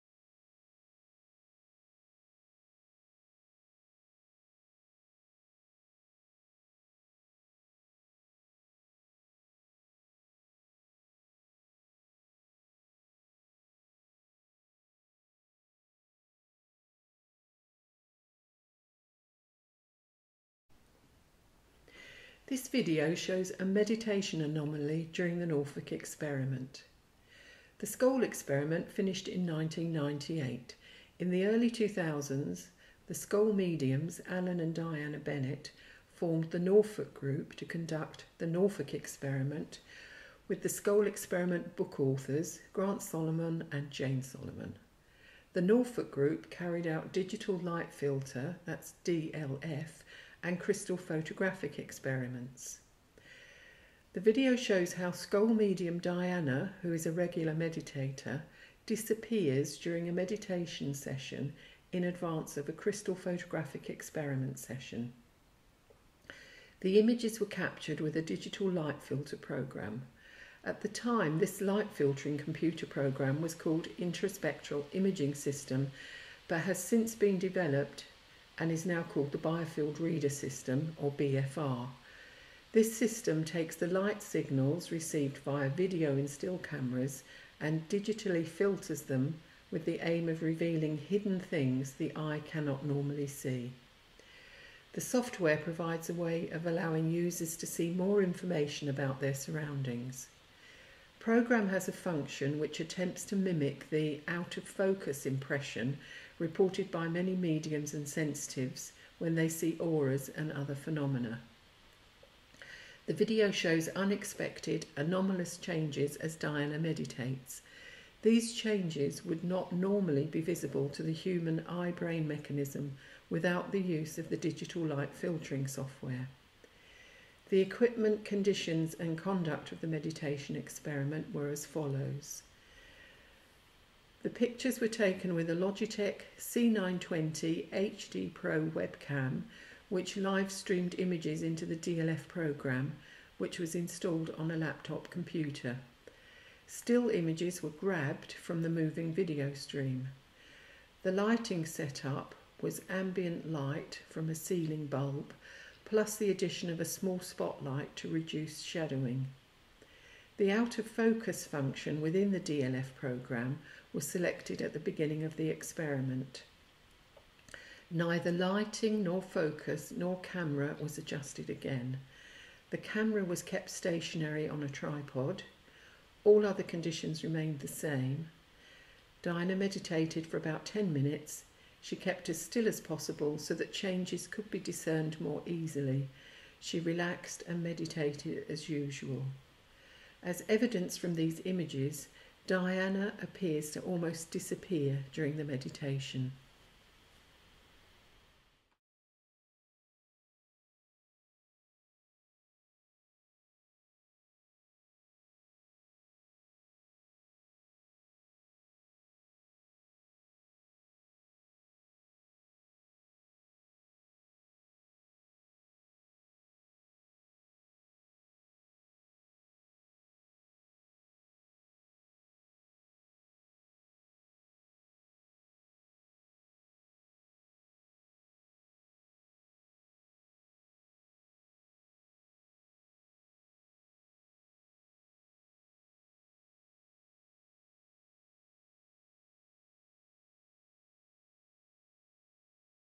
Scole Experiment medium disappears on screen during meditation
The Scole Experiment finished in 1998. In the early 2000s, the Scole mediums, Alan and Diana Bennett, formed the Norfolk Group to conduct The Norfolk Experiment with The Scole Experiment book authors, Grant Solomon and Jane Solomon. The Norfolk Group carried out digital light filter (DLF) and crystal photographic experiments.

This video from this 2013 experiment shows how Scole medium, Diana, who is a regular meditator, 'disappears' during a meditation session in advance of a Crystal Photographic Experiment session,

The images were captured with a digital light filter program. This light-filtering computer program is called Biofield Reader or BFR. The system takes the light signals received via video and still cameras and digitally filters them with the aim of revealing “hidden” things the eye cannot normally see. The software provides a way of allowing users to see more information about their surroundings. The program has a function which attempts to mimic the “out-of-focus” impression reported by many mediums and sensitives when they see auras and other phenomena.

The video of the experiment shows unexpected, anomalous changes as Diana meditates. These changes would not normally be visible to the human eye-brain mechanism without the use of the digital light filtering software.

Meditation Experiment

The pictures were taken with a Logitech C920 HD Pro Webcam which live-streamed images into the BFR digital light filter program, which was installed on a laptop computer. Still images were “grabbed” from the moving video stream. The lighting set-up was ambient light from a bulb in the ceiling, plus the addition of a small spotlight (to reduce 'shadowing'). The “out-of-focus” function within the BFR program was selected at the beginning of the experiment. Neither lighting, nor focus, nor camera was adjusted again. The camera was kept stationery on a tripod. All other conditions remained the same. Diana meditated for about 10 minutes. She kept as still as possible so that changes could be discerned more easily. She relaxed and meditated as usual.

Result of Meditation Experiment

As evidenced from these images, Diana appears to almost “disappear” on the computer screen during the meditation. We cannot currently explain the changes captured by the equipment and consider this to be an anomaly worth further investigation with the Biofield Reader (BFR) software.

Scole Group mediums’ comments

Diana said afterwards: “I was preparing for the crystal photographic experiment and my thoughts at the time were focused on it. I was making a connection with Spirit for the work we were about to do. I asked Spirit if it was possible that a change could be captured on the imaging equipment.”

Alan said: “During the  meditation, I was amazed at what I could see on the computer screen when I compared it with what I could see with my eyes. Diana was sitting normally in front of the camera but she was disappearing on the screen.”